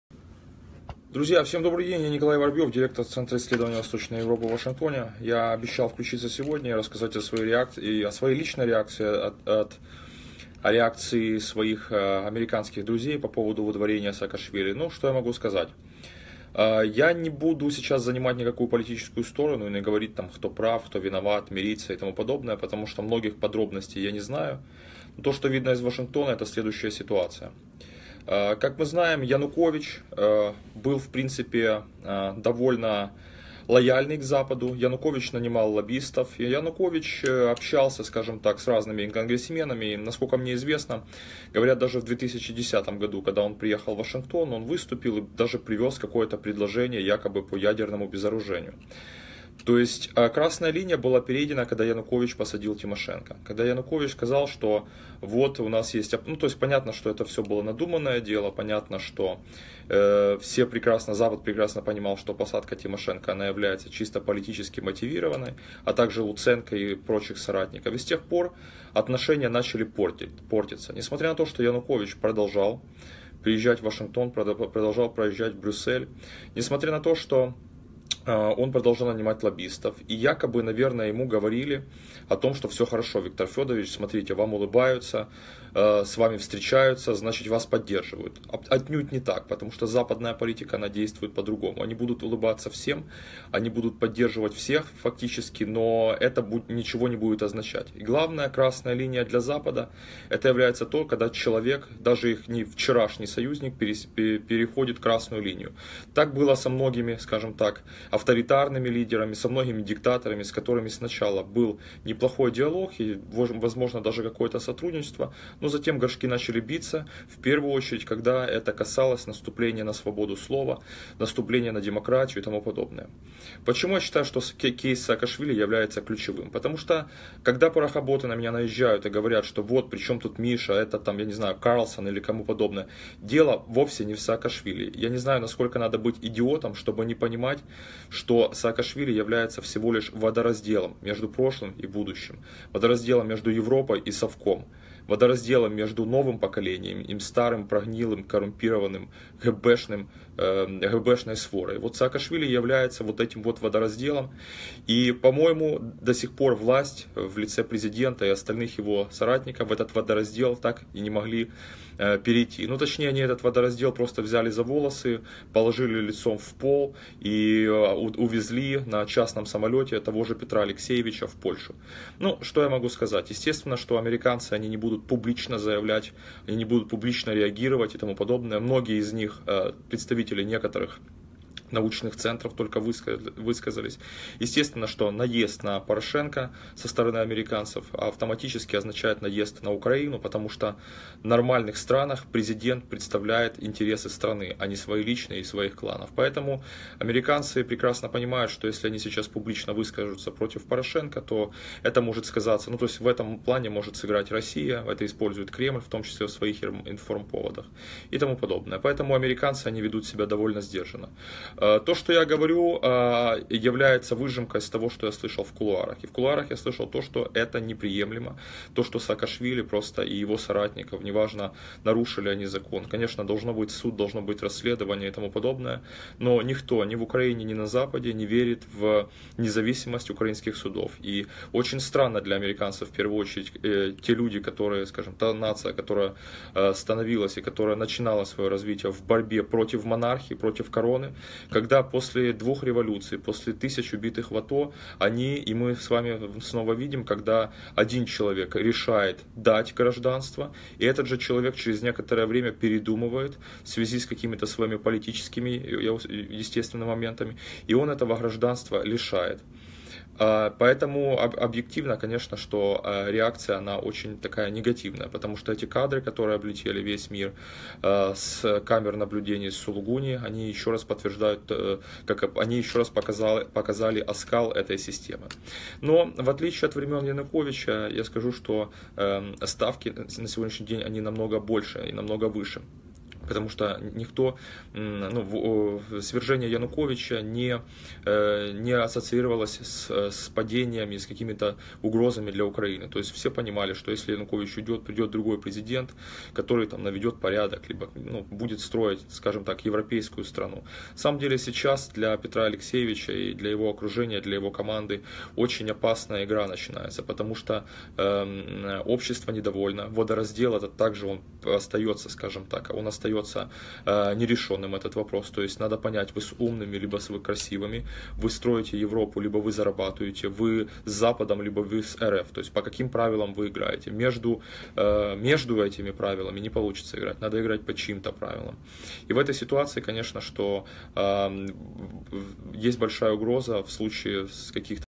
Реакция Вашингтона на выдворение Саакашвили. Простые пояснения о последствиях.
🗽Center for Eastern European Perspectives🗽